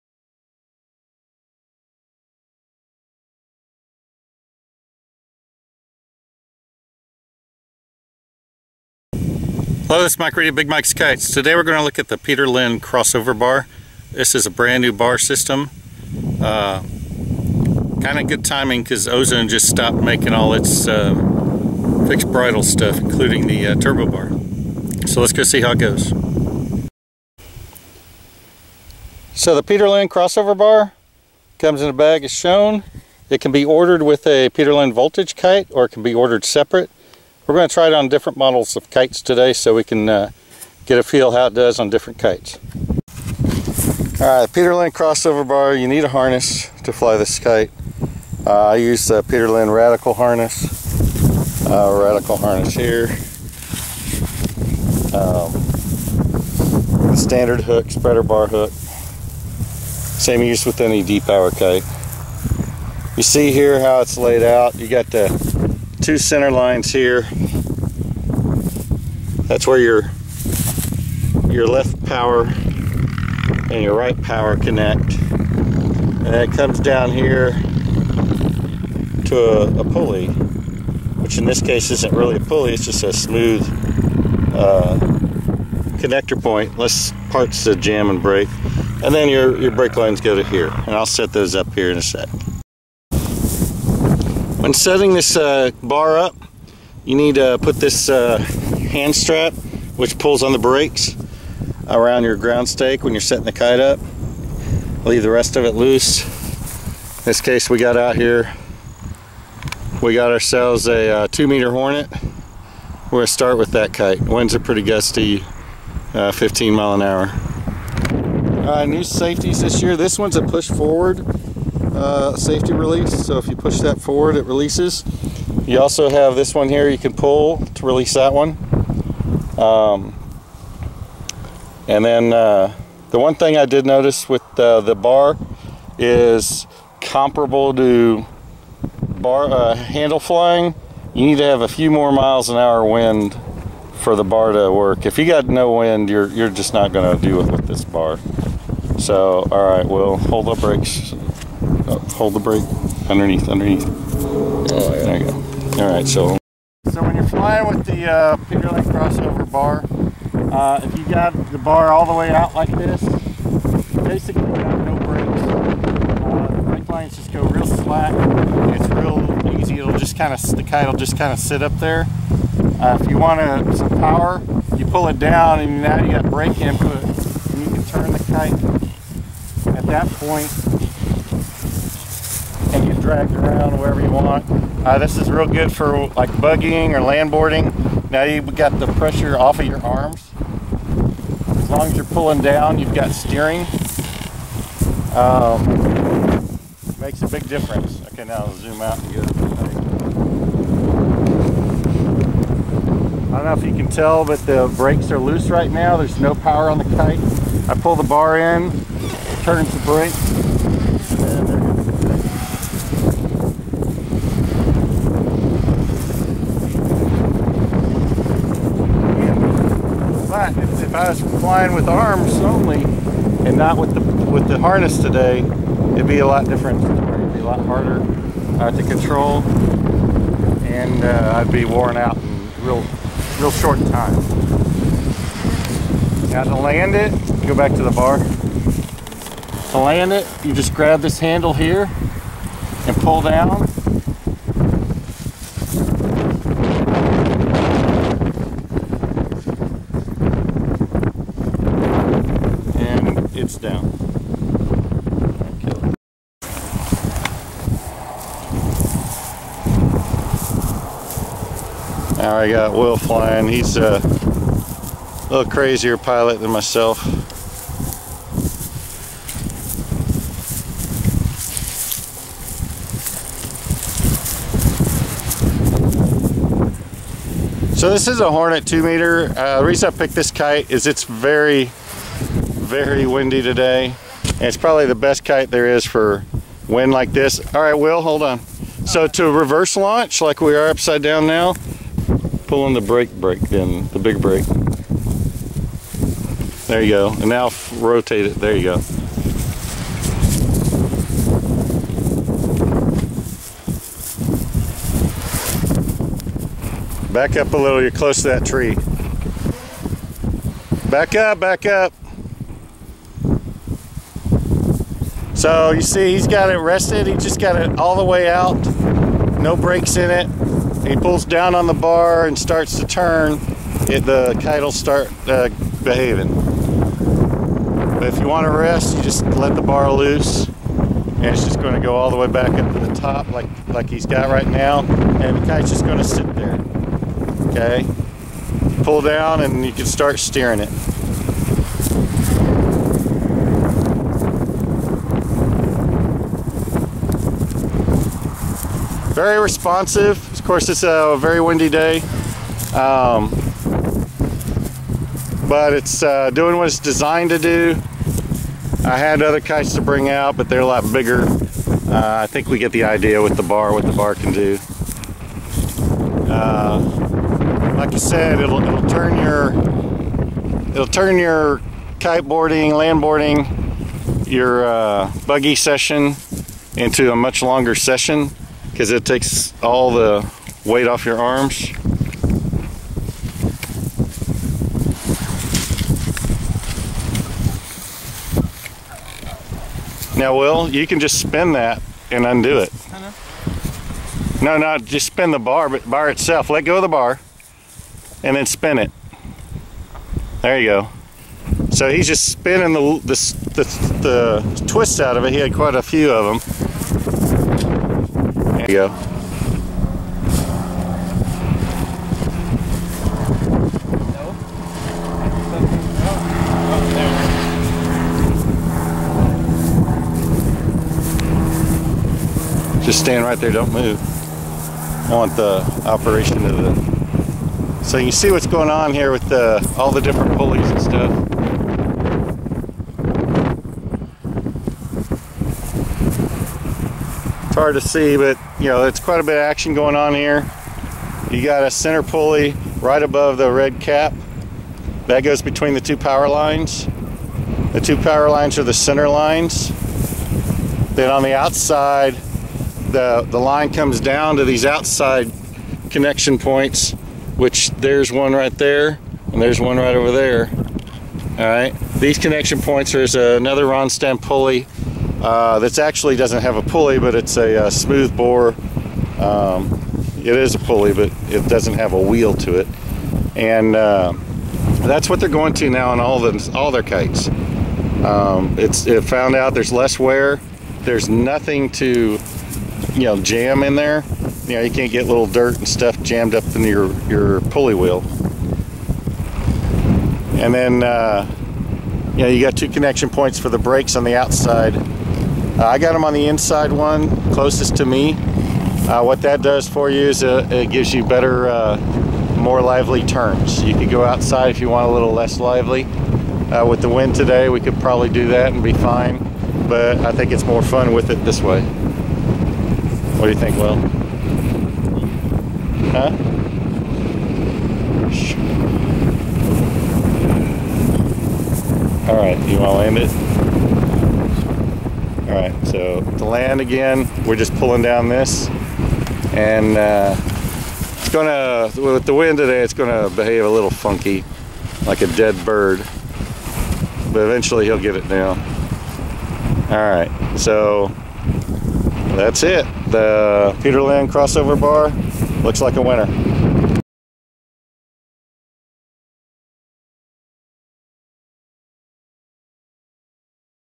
Peter Lynn CrossOver Bar for fixed bridle kites
The new Peter Lynn Crossover bar for 4 line fixed bridle kites.

Makes it much easier to landboard or kite buggy as it takes all the pressure off of your arms when flying.